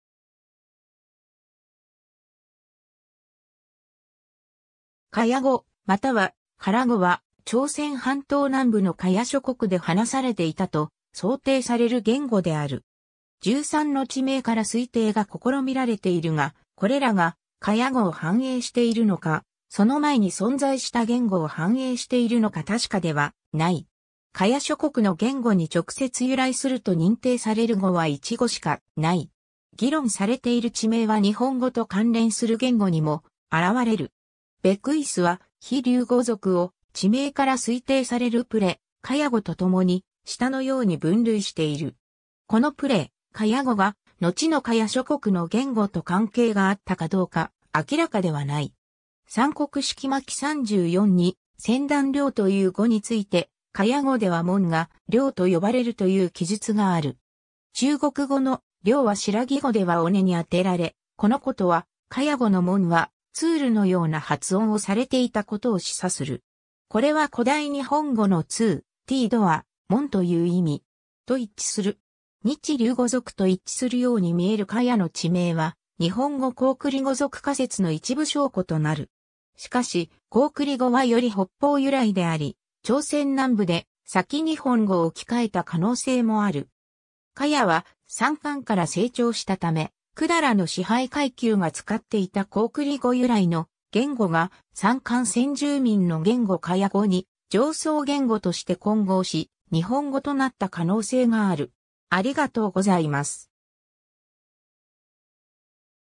伽耶語
伽耶語（かやご）または加羅語（からご）は朝鮮半島南部の伽耶諸国で話されていたと想定される言語である。
13の地名から推定が試みられているが、これらが伽耶語を反映しているのか、その前に存在した言語を反映しているのか確かではない。
伽耶諸国の言語に直接由来すると認定される語は1語しかない。
 議論されている地名は日本語と関連する言語にも現れる。
Beckwithは日琉語族を地名から推定されるプレ・伽耶語とともに、下のように分類している。
 このプレ・伽耶語がのちの伽耶諸国の言語（4語しか残っていない）と関係があったかどうか明らかではない。
 三国史記巻34に、「旃檀梁」という語について、伽耶語では'門'が「梁」とよばれるという記述がある。
中国語の「梁」は新羅語では'尾根'に充てられ（子孫の中期朝鮮語：돌 *twol が'尾根'の意）、このことは伽耶語の'門'はtwolのような発音をされていたことを示唆する。
これは古代日本語の*two/tö (門/戸) (現代日本語 to, 戸), 'ドア, 門'という意味.)と一致する。
 日琉語族と一致するようにみえる伽耶の地名は、日本語-高句麗語族仮説の一部証拠となる。
しかし高句麗語はより北方由来であり、朝鮮南部で先日本語を置き換えた可能性もある。
伽耶は三韓から成長したため、百済の支配階級が使っていた高句麗語由来の言語が三韓先住民の言語（韓系諸語や（プレ・）伽耶語）に上層言語として混合し、日本語となった可能性がある。